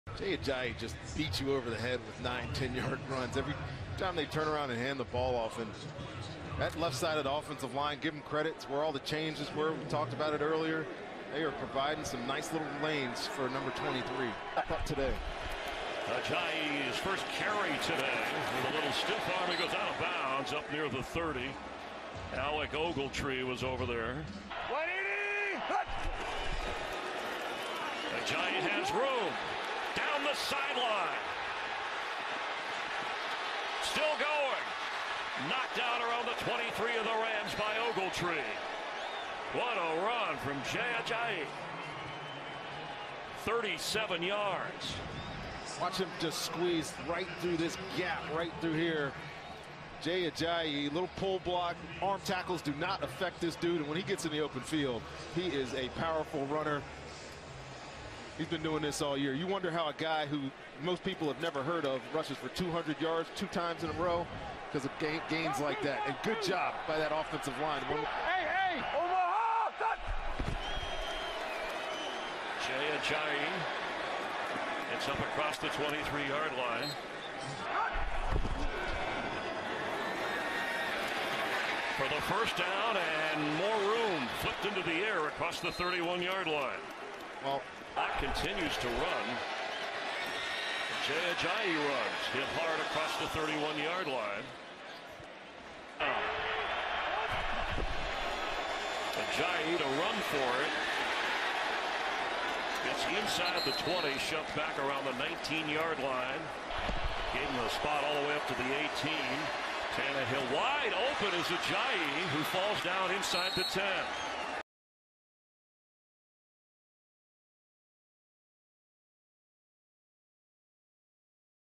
Jay Ajayi Highlights | Dolphins vs. Rams | NFL Week 11 Player Highlights
Jay Ajayi ran for 77 yards on 16 carries in Week 11 against the Los Angeles Rams.